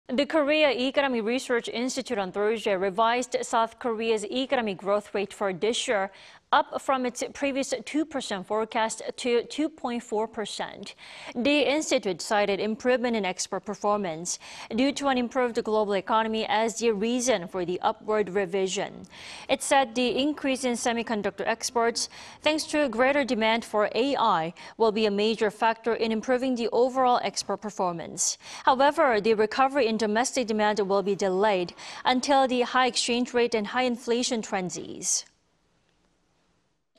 KERI revises up S. Korea's 2024 growth rate from 2.0% to 2.4%
한경연, 올해 경제성장률 2.0%→2.4% 상향 조정

The Korea Economic Research Institute on Thursday revised South Korea's economic growth rate for this year up from its previous 2-percent forecast to two-point-four percent.
The institute cited improvement in export performance due to an improved global economy as the reason for the upward revision.
It said the increase in semiconductor exports thanks to greater demand for artificial intelligence will be a major factor in improving the overall export performance.
However, the recovery in domestic demand will be delayed until the high exchange rate and high inflation trends ease.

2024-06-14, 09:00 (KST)